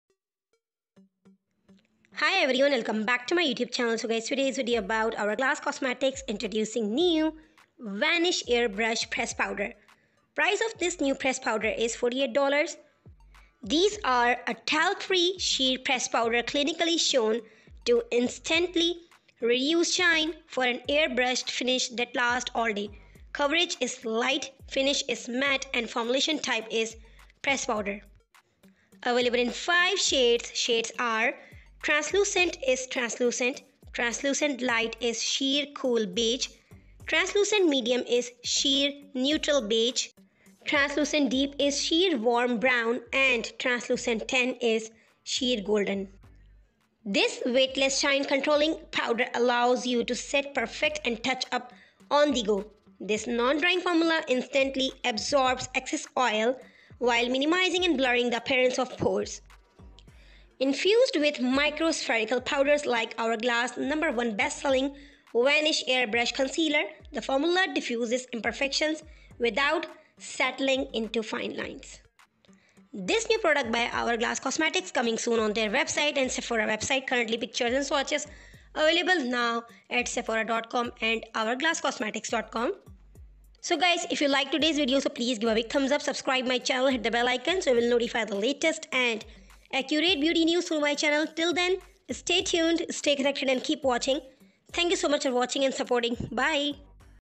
ComingSoon New!Hourglass Cosmetics Vanish Airbrush Pressed Powder|New Makeup Releases 2024
What it is: A talc-free, sheer, pressed powder clinically shown to instantly reduce shine for an airbrushed finish that lasts all day.

This weightless, shine-controlling powder allows you to set, perfect, and touch up on the go. This non-drying formula instantly absorbs excess oil while minimizing and blurring the appearance of pores. Infused with microspherical powders like Hourglass’ number-one bestselling Vanish Airbrush Concealer, the formula diffuses imperfections without settling into fine lines.

Translucent - translucent
Translucent Tan - sheer golden
Translucent Light - sheer cool beige
Translucent Deep - sheer warm brown
Translucent Medium - sheer neutral beige

Price of each $48